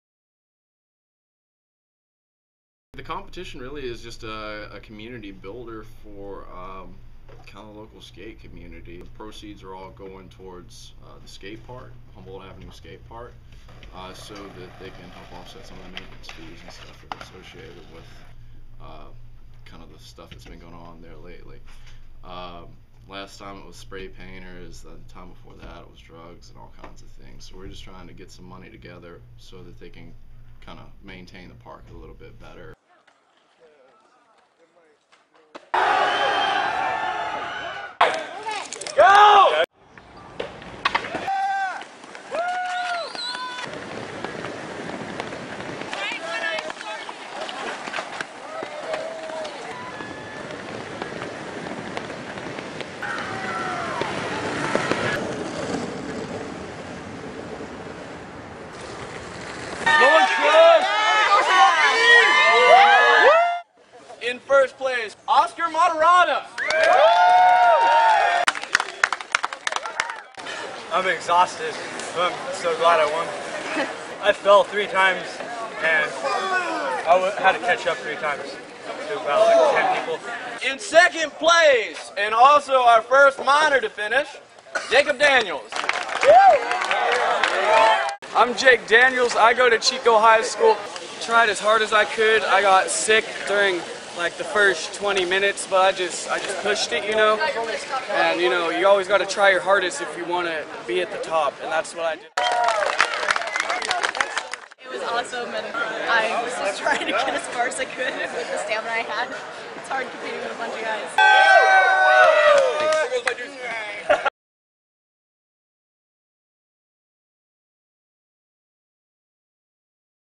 Chico Big Push
The longboard race through Bidwell Park began at One Mile, near South Park Drive, and continued down the bike path through Bidwell Park on Saturday, September 3.

Boarders crossed the finish line near Sycamore Pool. Tickets to race cost $10, and the first to finish won a $1,000 cash prize.

CJ Carmichael, manager of Boards on Nord, created Chico Big Push in efforts to raise money and awareness for cleaning up Humboldt Neighborhood Park, Chico's only skatepark.

Produced by Joey Angelotti, Gary Angell and Ashley Nakano - The Orion 2011